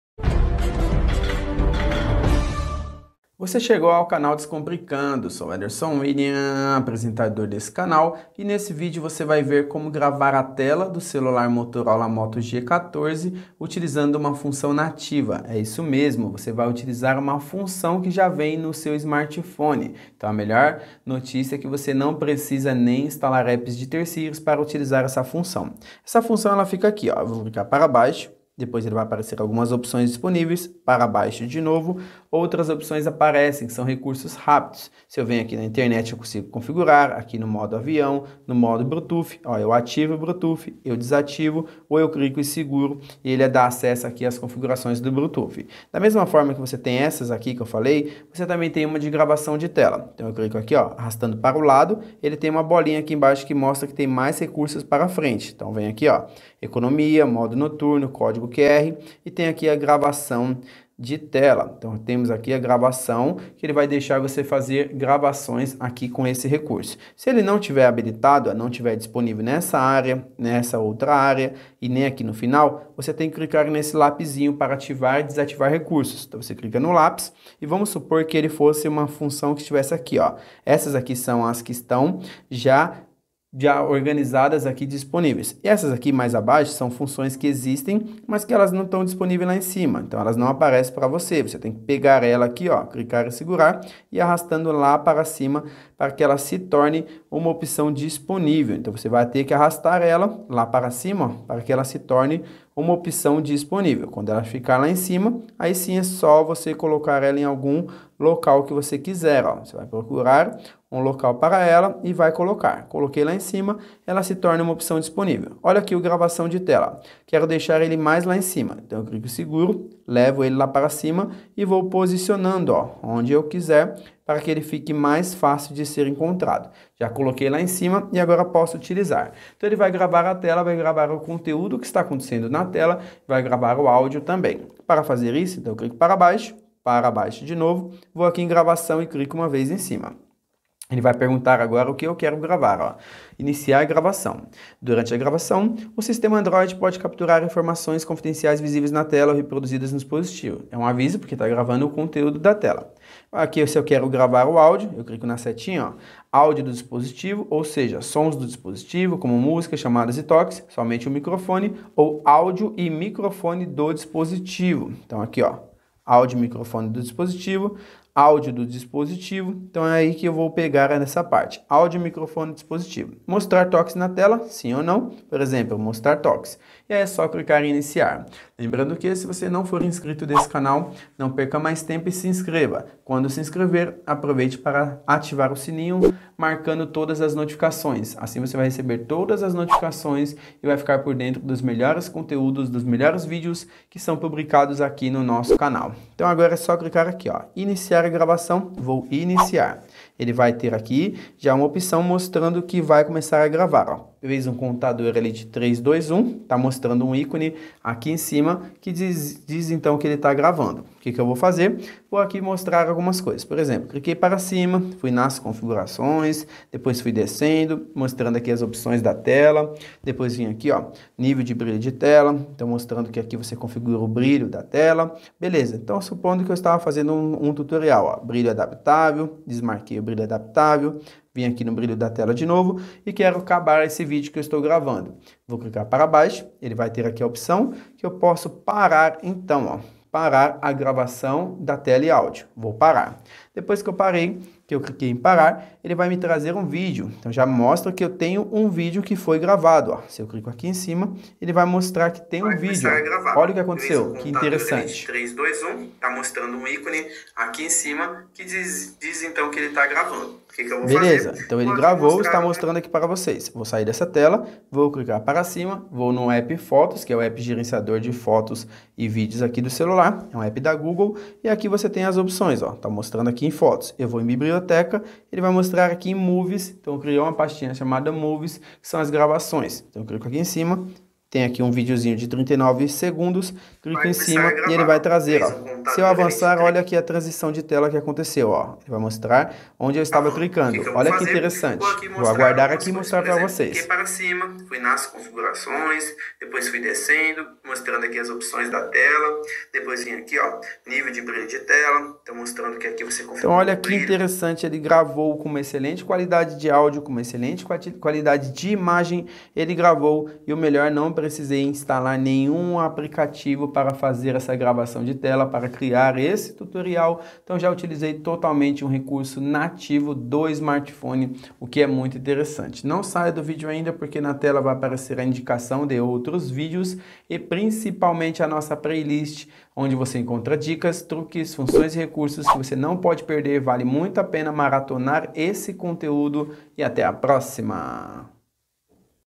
COMO GRAVAR A TELA DO CELULAR MOTOROLA MOTO G14 (FUNÇÃO NATIVA)
Aprenda a melhor maneira de gravar um conteúdo da tela do seu smartphone Motorola Moto G14 através de uma funcionalidade nativa, que pode ser muito interessante. Muitas pessoas desejam ter um aparelho com essa função de maneira padrão, o que quer dizer que não é necessário baixar e instalar Apps de terceiros para fazer o que o telefone já é capaz de fazer, e felizmente alguns aparelhos da Motorola oferecem essa opção. Assim, sempre que quisermos gravar um tutorial, um passo a passo, uma explicação ou simplesmente salvar um conteúdo de maneira animada na galeria isso será possível e muito fácil.

Além disso, gravar a tela do nosso celular será um ótimo meio de capturar todas as atividades que ocorrem na tela do smartphone em tempo real, assim, isso pode incluir a nossa interação com amigos em Apps mensageiros, aplicativos, jogos, navegação na internet, vídeos de diversas plataformas que permitem fazer gravação ou qualquer outra coisa exibida no display do dispositivo. Por isso, para saber como fazer isso basta seguir o passo a passo completo do nosso vídeo.

⬇Você pode gostar desses vídeos⬇

▶▶MOTOROLA MOTO G14: COMO REMOVER TODOS OS ATALHOS DE APPS E WIDGETS DA TELA INICIAL DO CELULAR